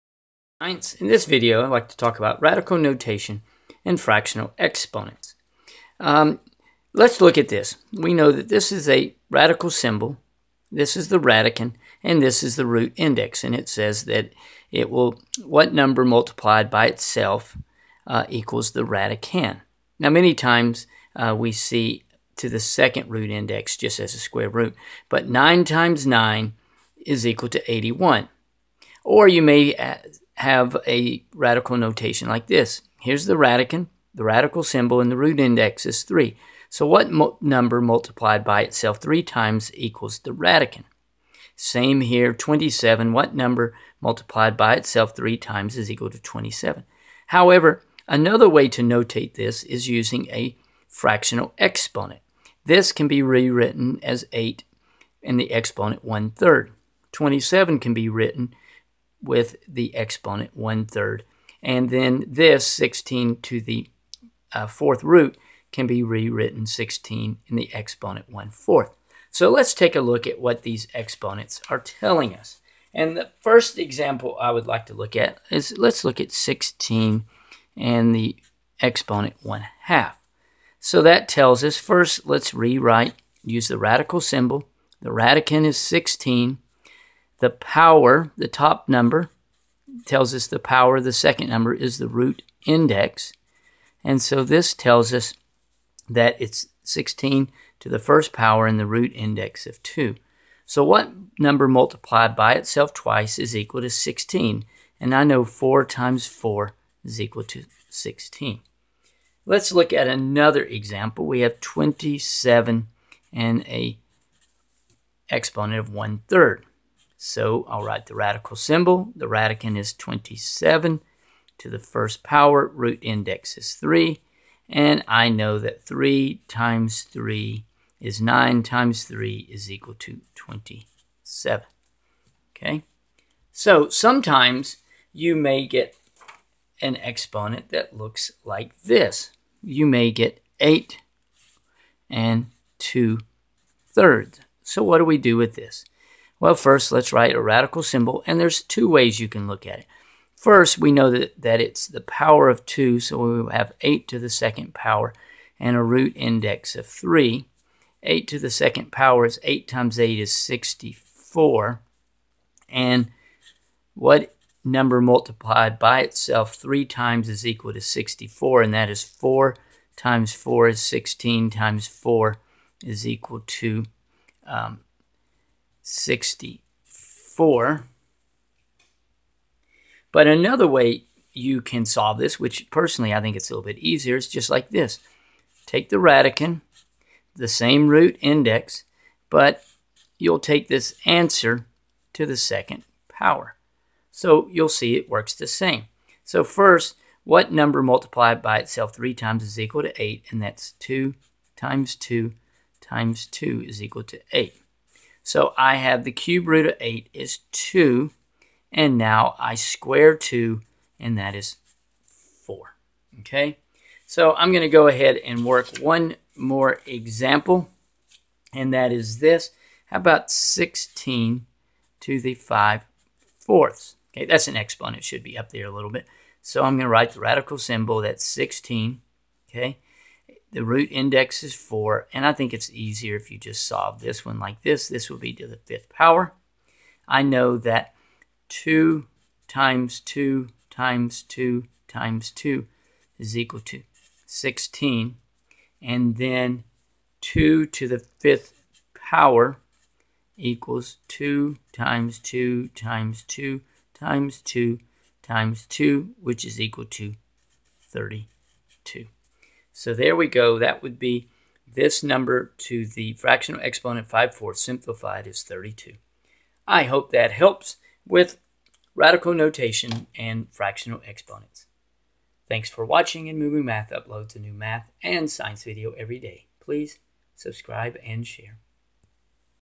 What do fractional exponents mean?
What do fractional exponents mean? Fractional exponents are also called rational exponents. They are another form of rational notation.
The numerator expresses the power and the denominator represents the root index.
The radicand is the number and the root index tells you how many times a number is multiplied by itself in order to equal the radicand.